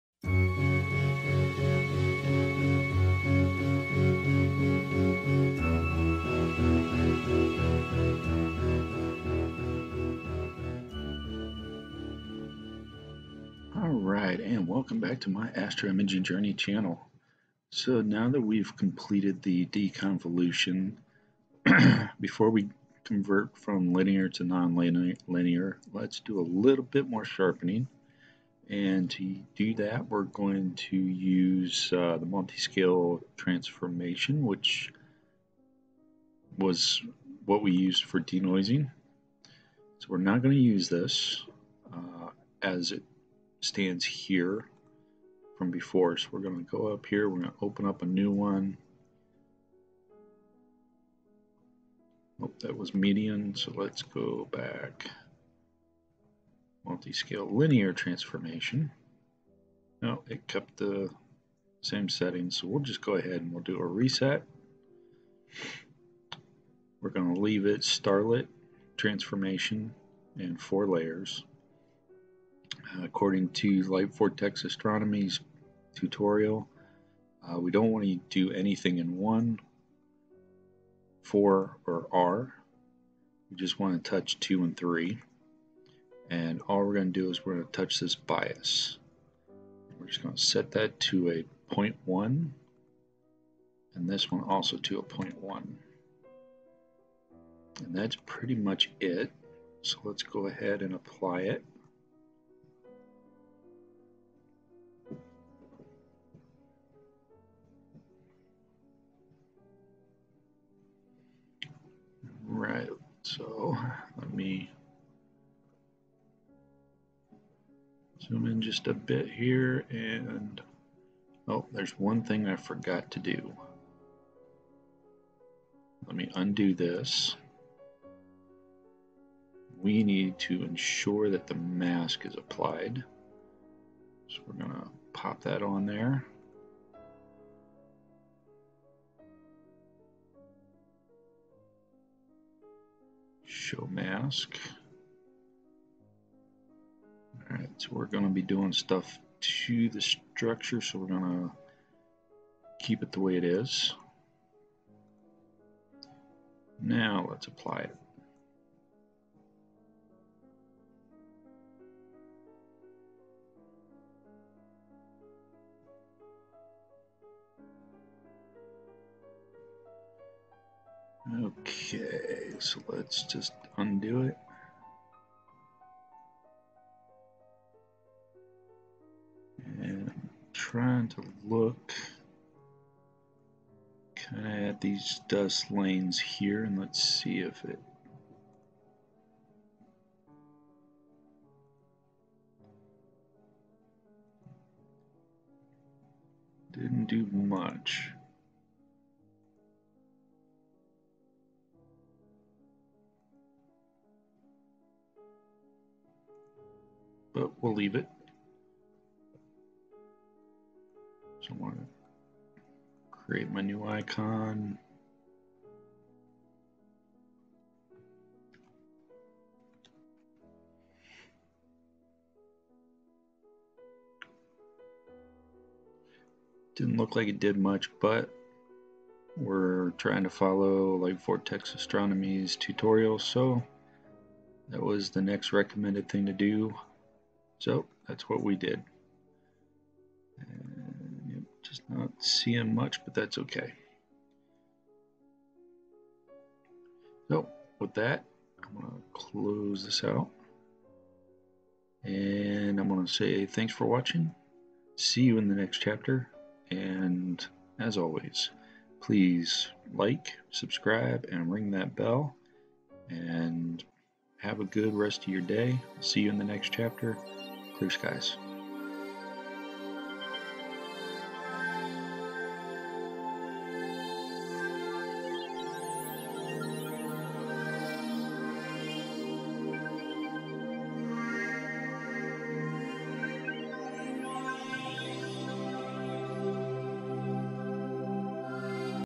EP51 CH27 Sharpening using the PixInsight MultiScaleLinearTransform Process
In this chapter I discuss the "MultiScaleLinearTransform" process.  This is part of a PixInsight workflow based on the information provided on the Light Vortex Astronomy tutorial website.

Celestron CGEMII mount
Canon T6i
Orion 60mm Guide Scope
Orion StarShoot
Thousand Oaks 4-channel dew heater
AV Access USB Extender